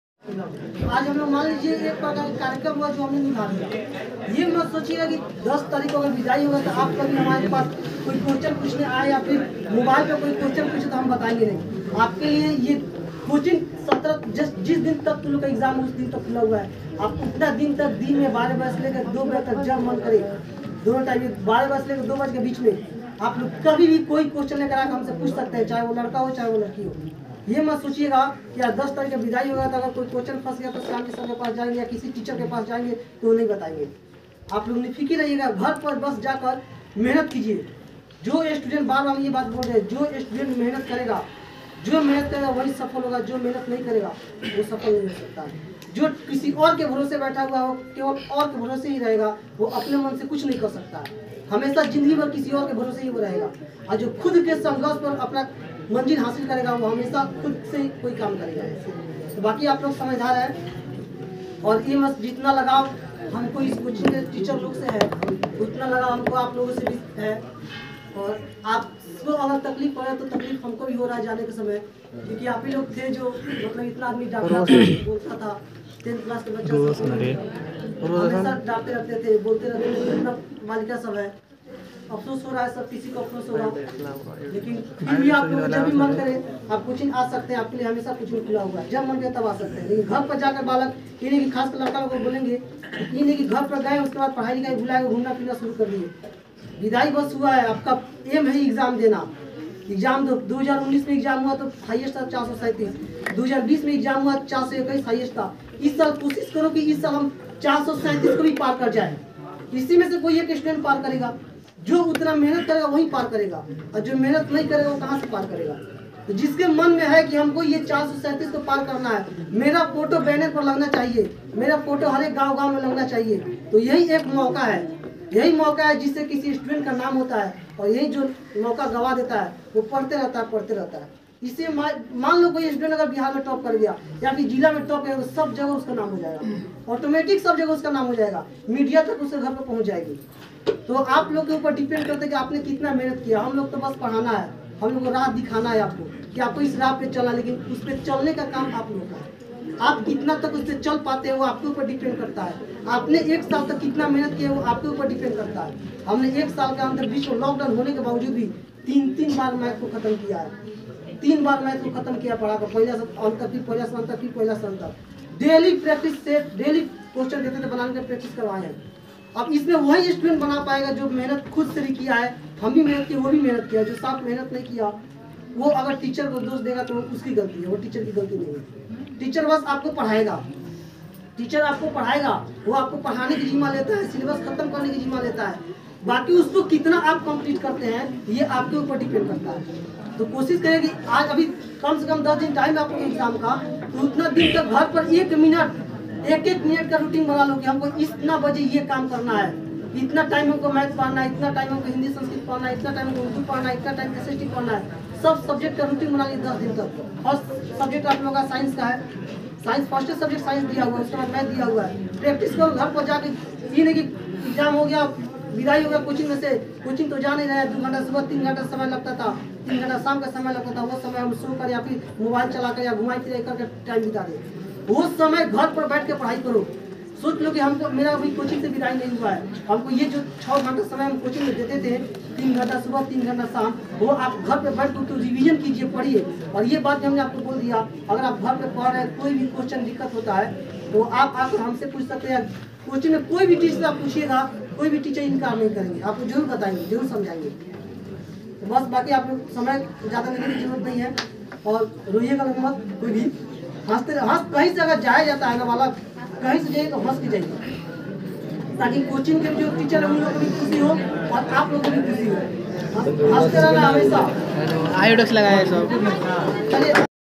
Farewell 10th class 2021in career point Ramnagar
Motivational speech for students by Shyam sir